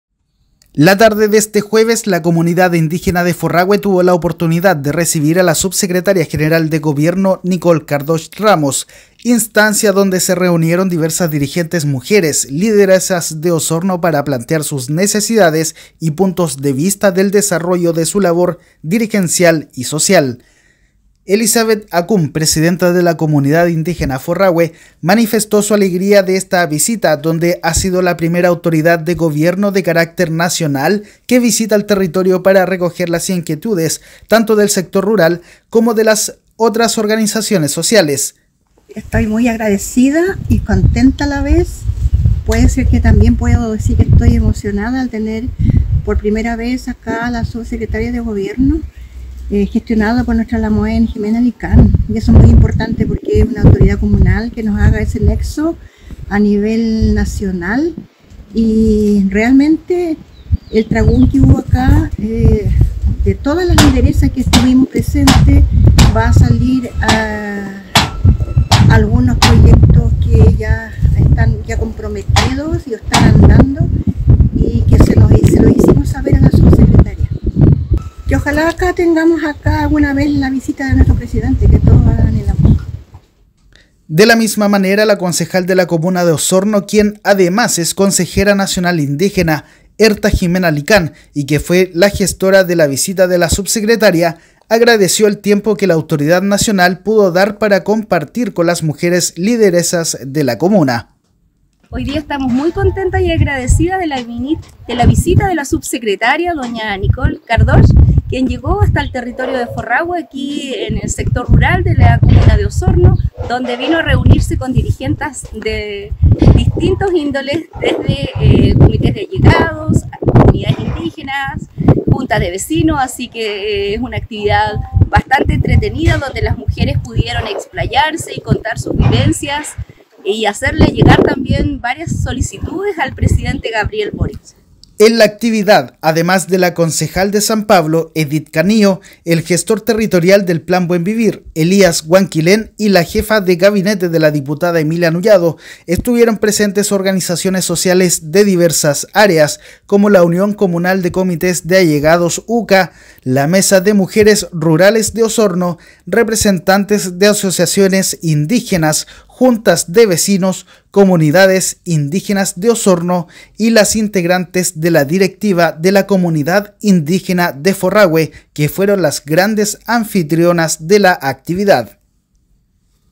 Mujeres lideresas de Osorno se reunieron con la subsecretaria general de gobierno.
La tarde de este jueves la Comunidad Indígena de Forrahue tuvo la oportunidad de recibir a la Subsecretaria General de Gobierno Nicole Cardoch Ramos, instancia donde se reunieron diversas dirigentes mujeres lideresas de Osorno para plantear sus necesidades y puntos de vista del desarrollo de su labor dirigencial y social.